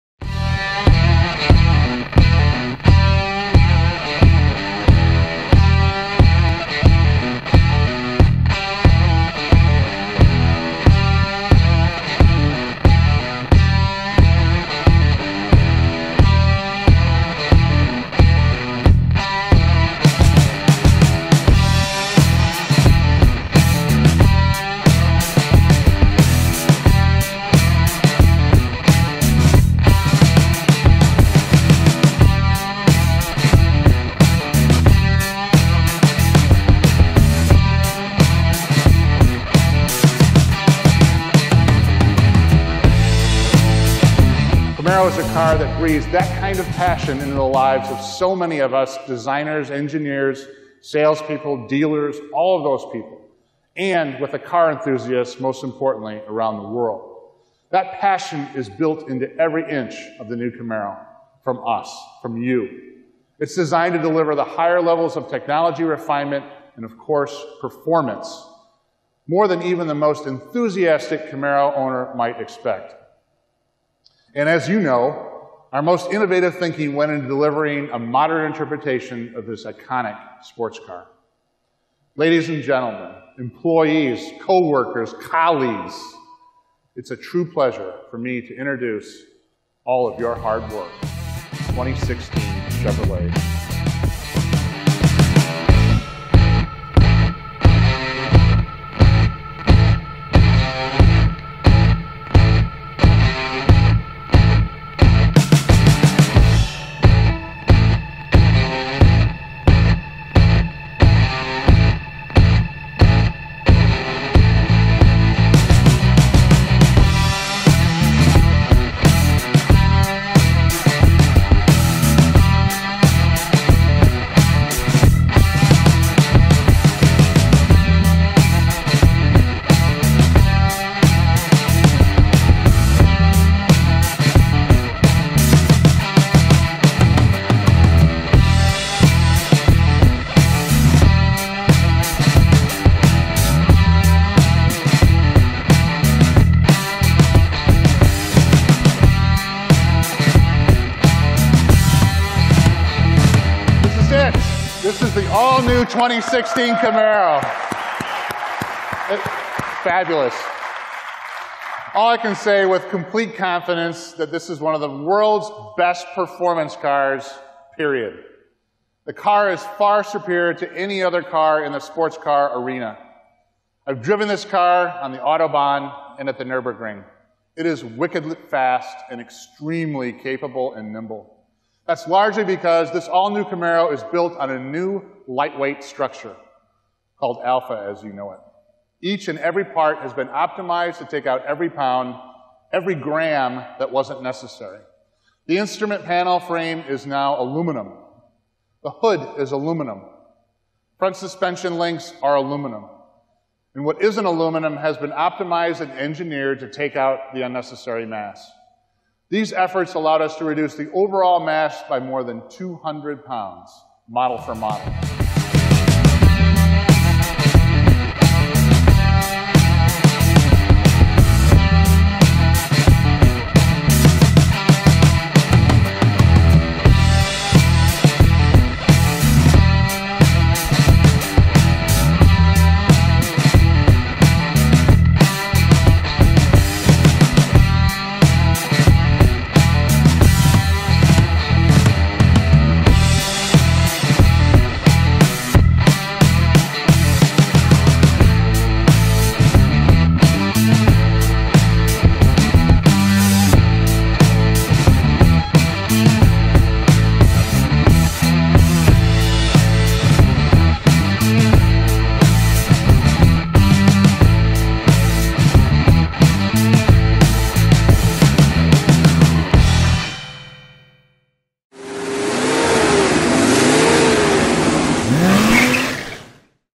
2016 Chevrolet Camaro Unveiled on Belle Isle, Detroit, MI | AutoMotoTV
The sixth-generation Chevrolet Camaro offers higher levels of performance, technology and refinement and is designed to maintain the sporty car segment leadership earned over the past years. The Gen Six Camaro provides a faster, more nimble driving experience, enabled by an all-new, lighter architecture and a broader powertrain range. Six all-new powertrain combinations are offered, including a 2.0L Turbo, an all-new 3.6L V-6 and the LT1 6.2L V-8, which is SAE-certified at 455 horsepower (339 kW) and 455 lb-ft of torque (617 Nm) - for the most powerful Camaro SS ever. Each engine is available with a six-speed manual or eight-speed automatic transmission.